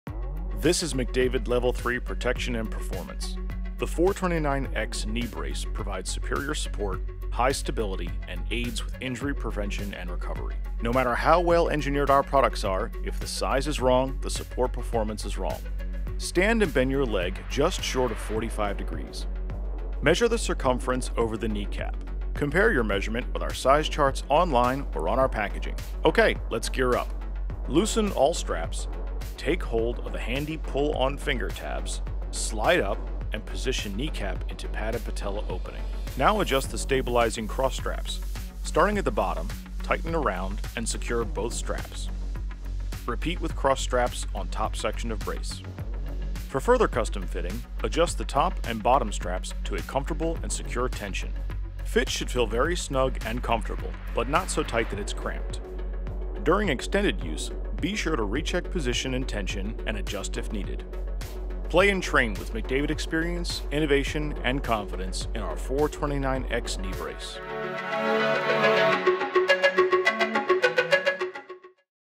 McDavid SportMed – How to Fit Knee Brace With Polycentric Hinges and Cross Straps (MD429X)
The McDavid 429X Knee Brace With Polycentric Hinges and Cross Straps delivers superior support, high stability and aids injury prevention and recovery. It provides Level 3 Maximum Protection — to ensure this level is right for you, watch this video and see how to fit the McDavid Knee Brace With Polycentric Hinges and Cross Straps.